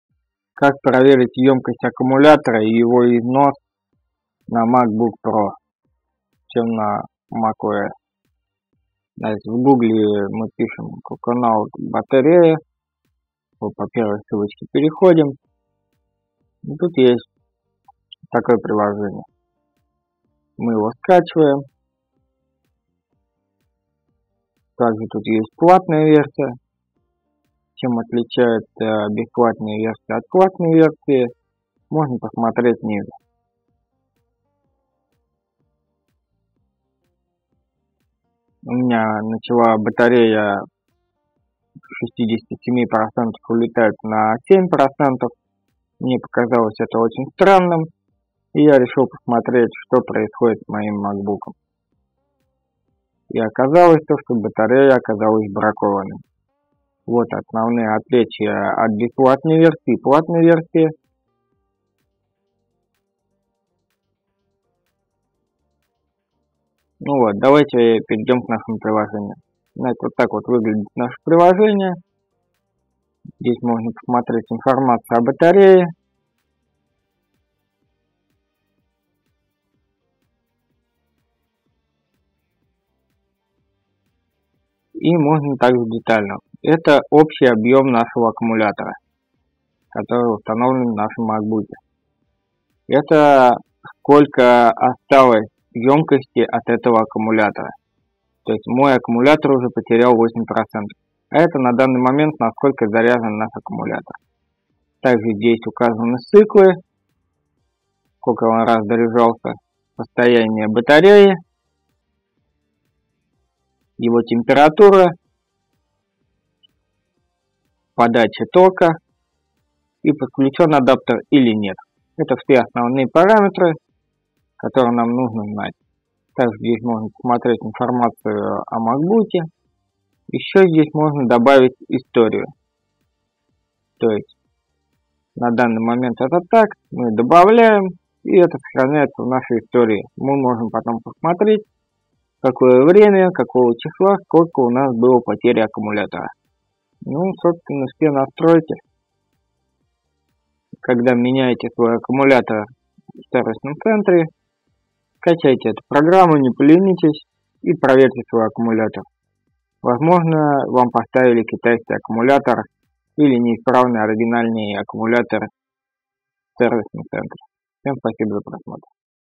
Как проверить аккумулятор на MacBook Pro (OS X)
Если у вас аккумулятор стал резко терять свою емкость, допустим с 67% улетать на 7%, то стоит проверить ваш аккумулятор в вашем MacBook Pro. Данная программа называется CoconutBattery и используется на операционной системы Mac OS X и показывает все характеристики вашего аккумулятора.